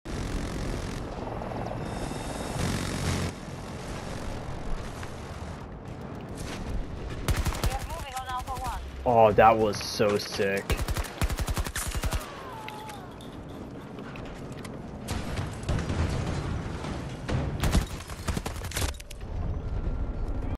Successfully failed wingsuit landing.
Chopper was lower than I expected when I jumped.